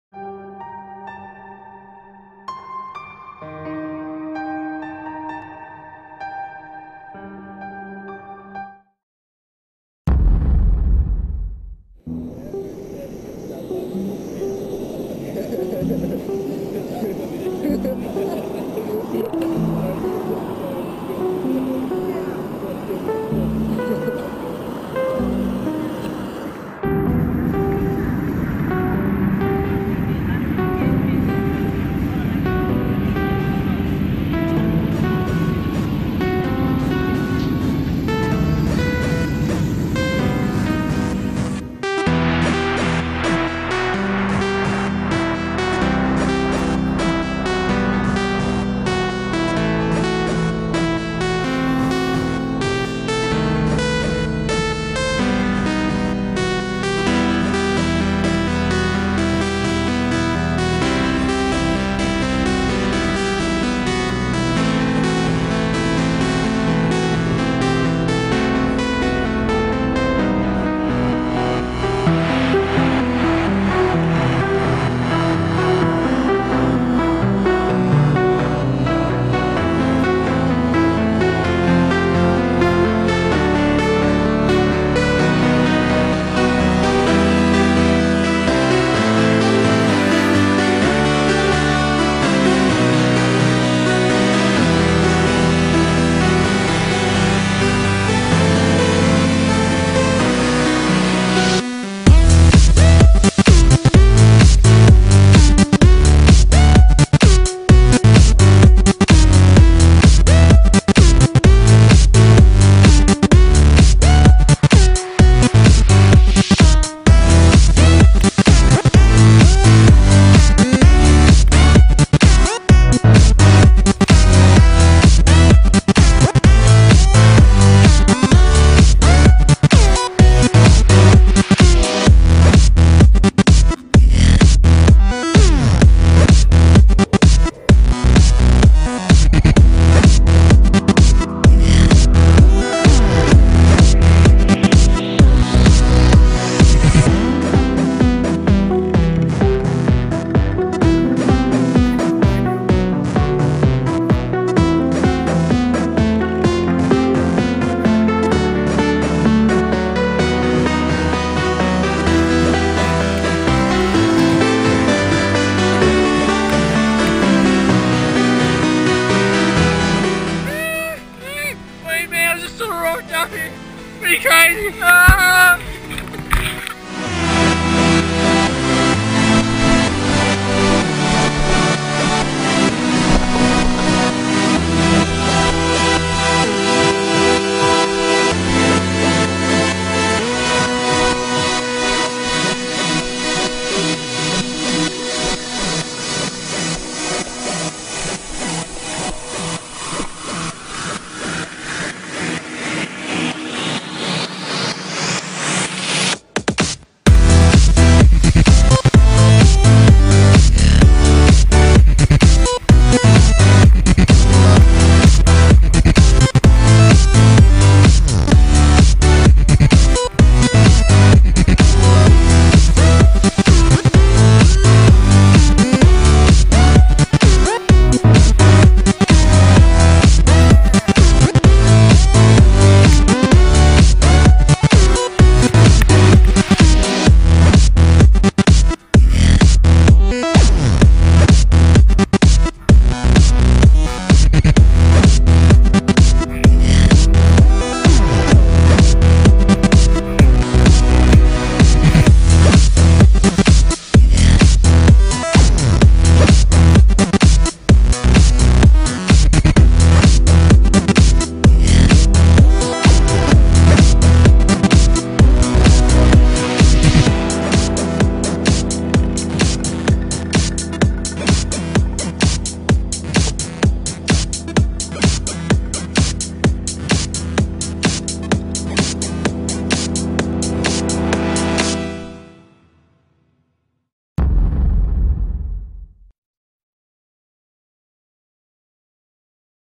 Ski Trip 2012
Shot 100% on the GoPro Hero 2

After a week of skiing (snowboarding) and another week of editing, the ski trip is finally ready to be showcased to our viewers, so here it is. Enjoy!